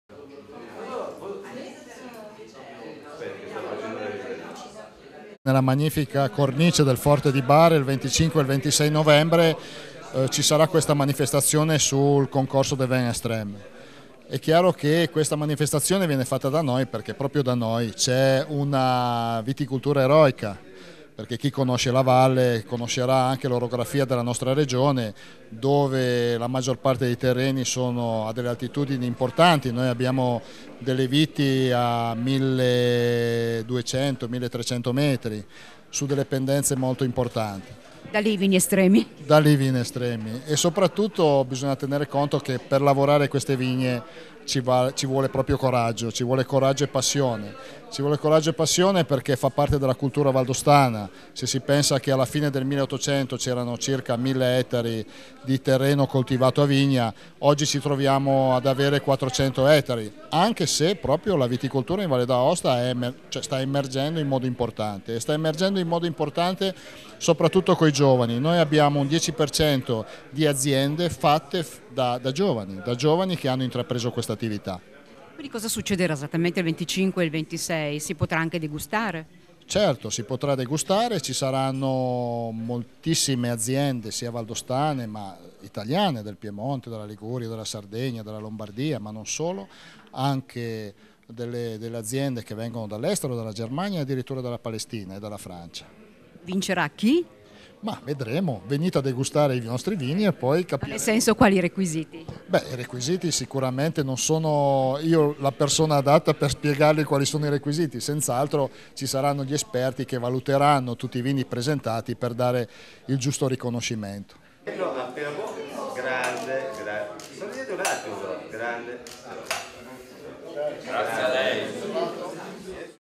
MONDIALE DEI VINI ESTREMI: VALLE D'AOSTA PREMIA PRODOTTI AUDACI DELLE TERRE SELVAGGE
Sette Gran medaglie d'oro, 110 medaglie d'oro e 103 medaglie d'argento assegnate durante la venticinquesima edizione del Mondial des Vins extremes, organizzato dal Cervim e dedicato alla viticoltura 'eroica' perchè praticata su terreni in forte pendenza. La premiazione è in programma il 26 novembre al Forte di Bard nell'ambito della seconda edizione della manifestazione Vins extremes. Le Gran medaglie d'oro sono state assegnate a vini di Toscana, Mosella (Germania), Isola di Samos (Grecia), Madeira (Portogallo), Douro (Portogallo) e Georgia. La presentazione nella sede di Unioncamere Piemonte a Torino